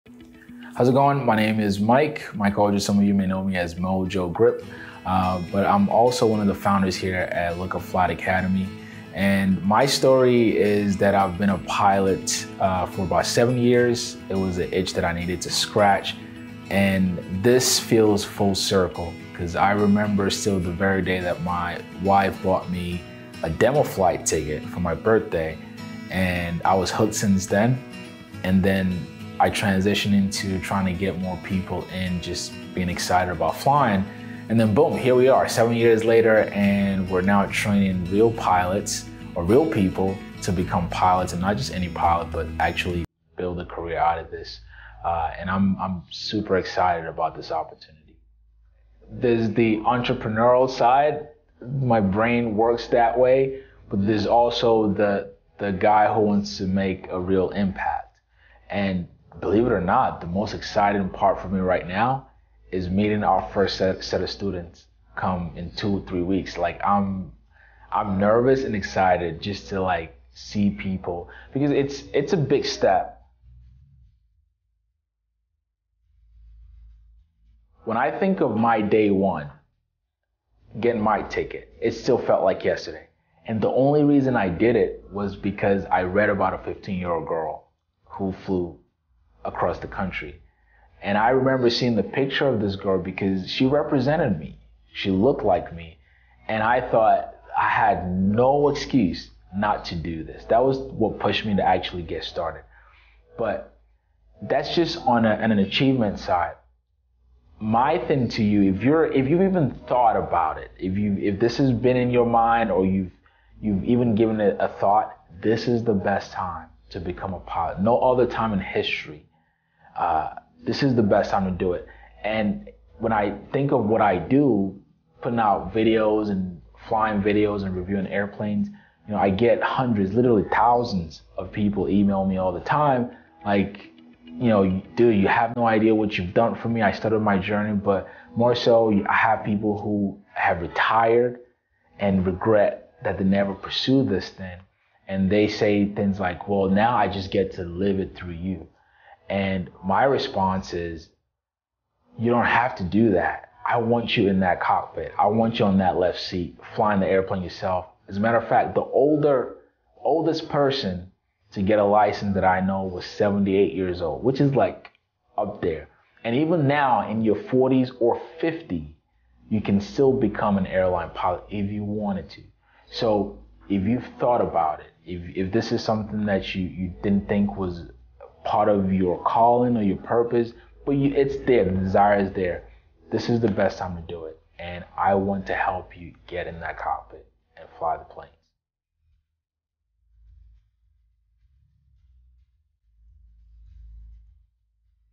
Why I Started My Own Pilot School
Fall of 2015 was the first time I ever flew an airplane as a potential pilot. I still remember that day like it was yesterday. Fast forward 8 years later, I have been able to get certified and met a lot of great people in the aviation industry. Now I am pretty much an evangelist for the pilot community.

Now more than ever is the best time in history to become a Pilot. Together with my partners we opened our flight school in Winston Salem NC Lookup flight academy where we train you from zero all the way to commercial pilot and get you ready for the airlines.